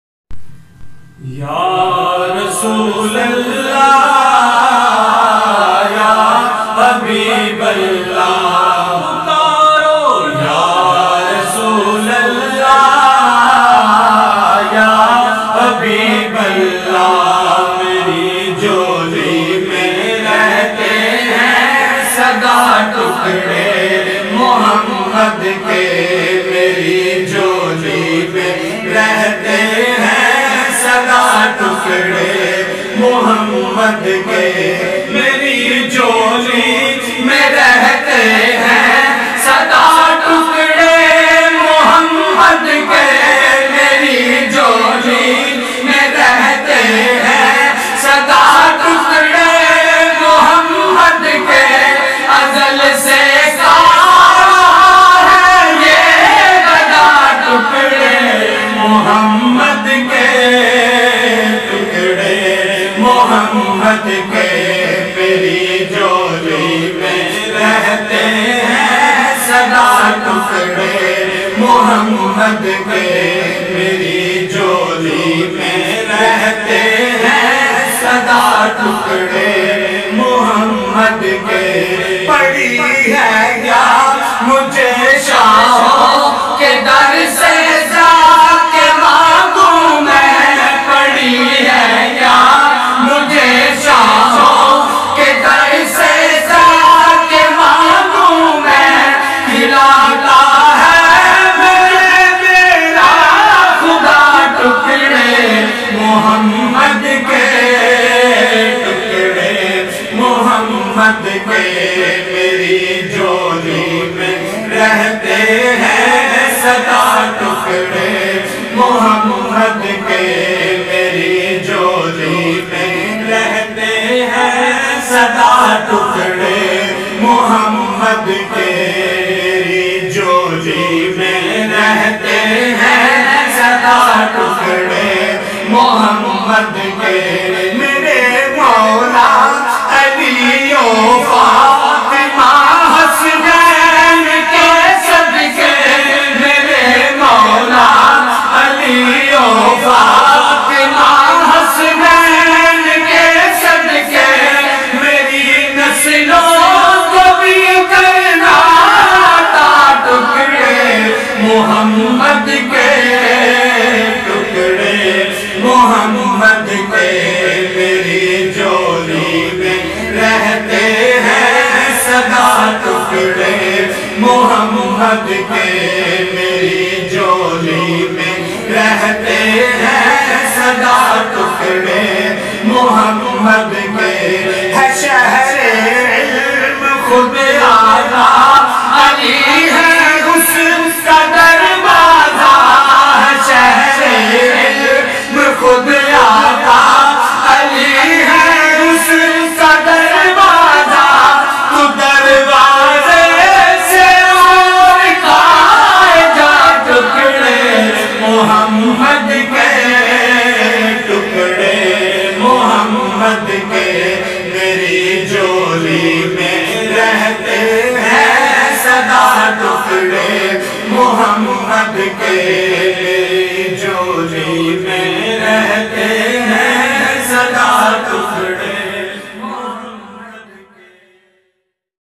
Zaheer chishti | naat khawan | naat rasool maqbool | zaheer chisti gujar khan
بلبل مصطفیٰ برادر ظہیر چشتی گوجرخان سے نعتیہ کلام اپنے بیٹے اور بزرگوں کے ساتھ مل خوبصورت انداز میں ہدیہ نعت پیش کرتے ہوئے...